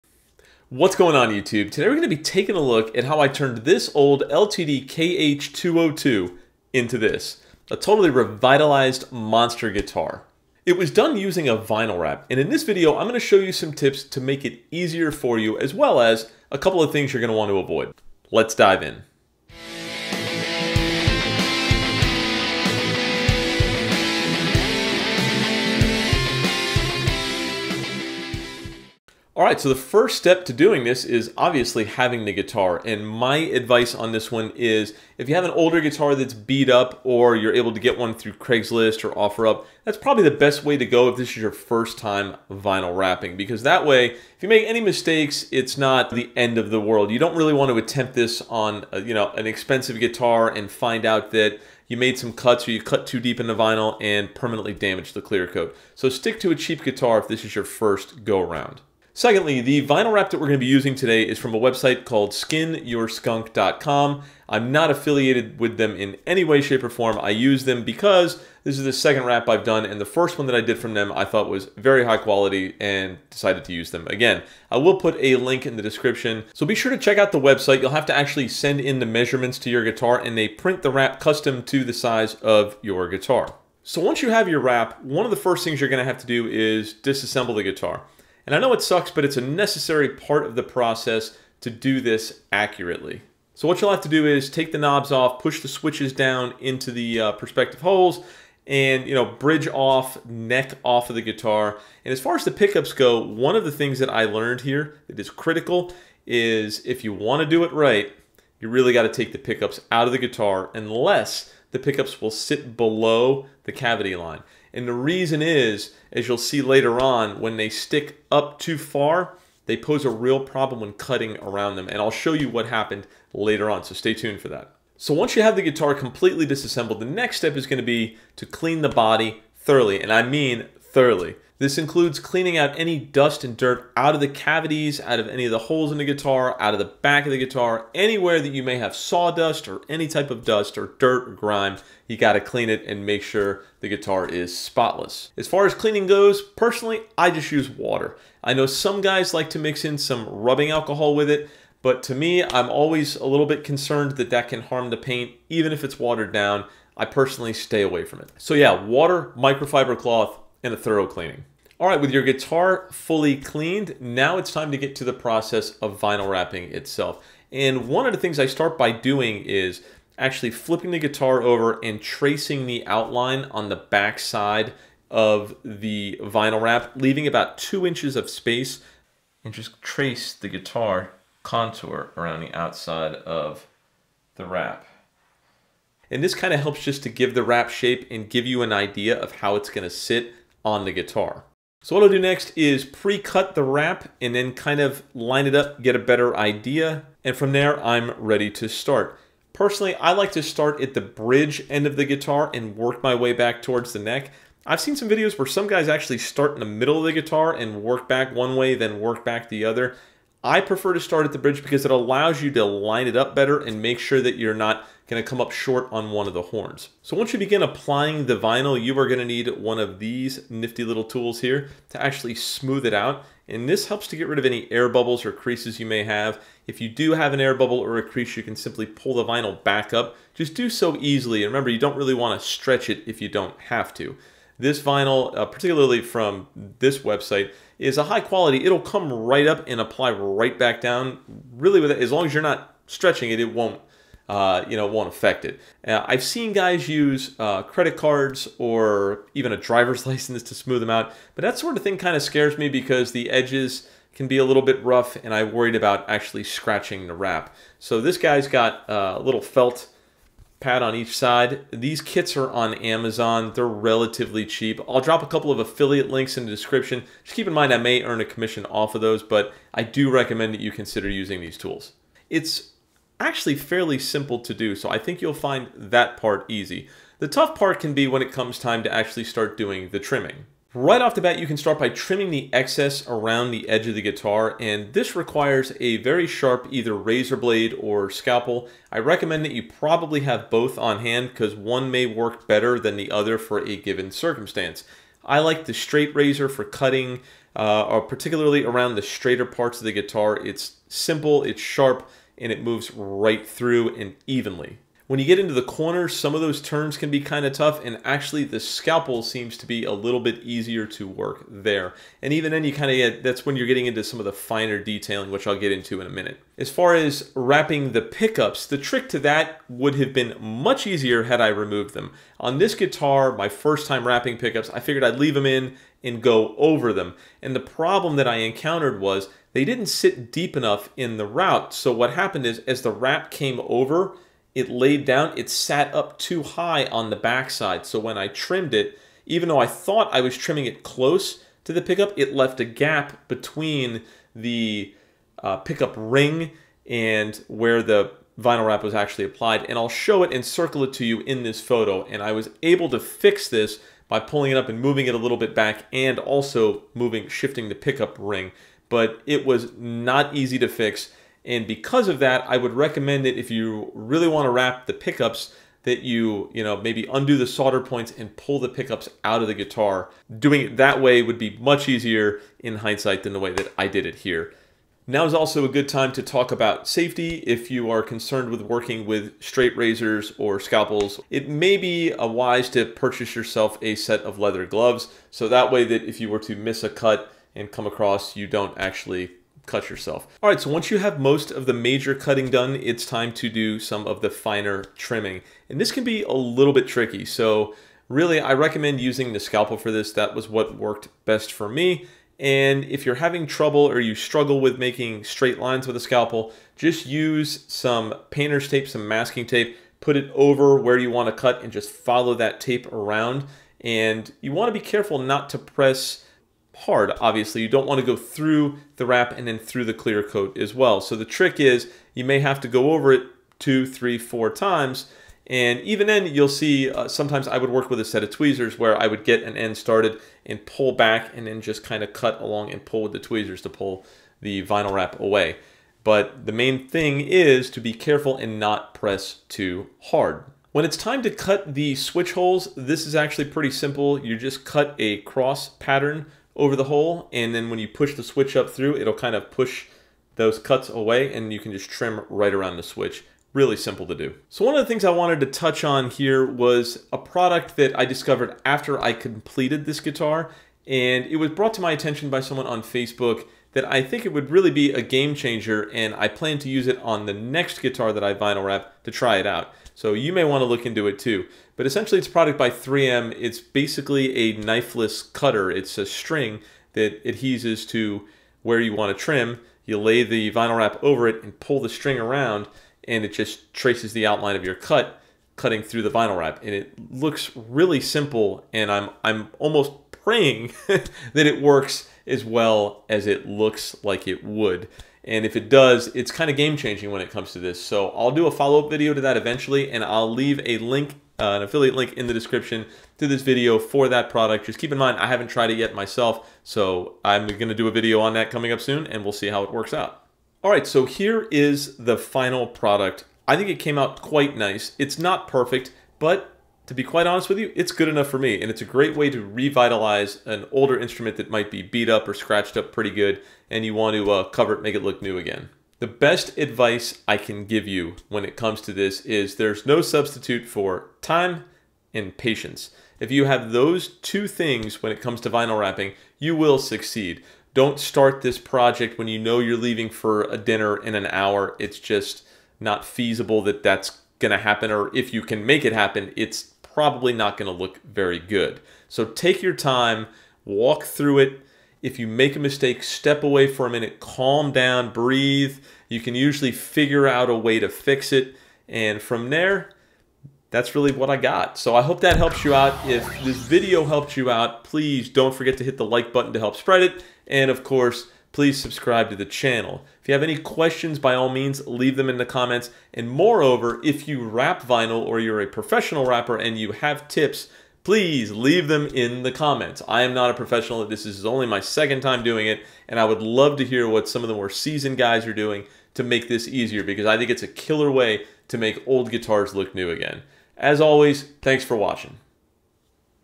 Making a MONSTER: Tips to vinyl wrap your Guitar
What's going on Youtube. A while back, I had an idea to create a monster guitar that KIRK HAMMETT didn't. So I decided to try my hand at vinyl wrapping and figured I'd try to get a hold of a KH-202. As luck would have it, I found one on OfferUp for $120 and the rest is history. I hope this video helps you along with vinyl wrapping your guitar. I didn't use a Heat Gun on this project because honestly, I didn't think it was necessary. If you have stronger curves in the guitar shape, you made need one. Always be sure to wear eye protection and gloves when using a blade or exacto knife as well, always use caution. Lastly, I'm no pro at it but I figured I'd share some tips that I learned along the way.

Vinyl Wrap Kit

3M DesignLine Knifeless Tape

Social